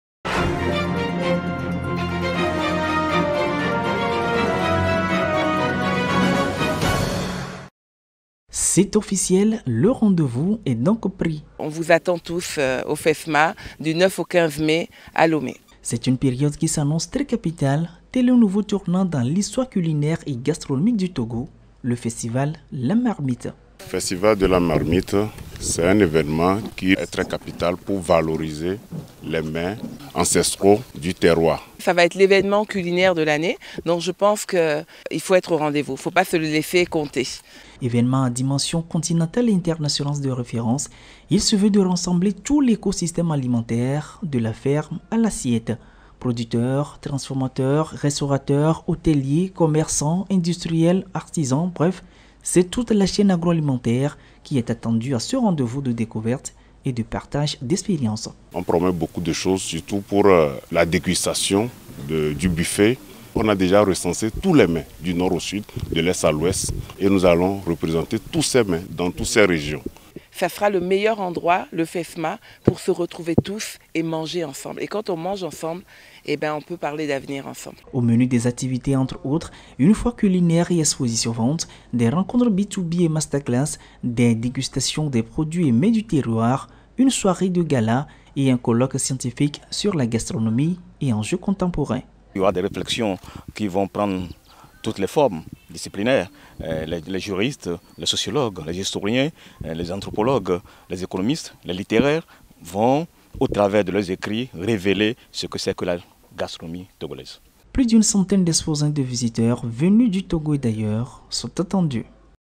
FESMA 2022: le rendez-vous par excellence de la gastronomie togolaise, bientôt
C'est une période qui s'annonce très capitale, tel un nouveau tournant dans l'histoire culinaire et gastronomique du Togo : le festival la marmite.@direct7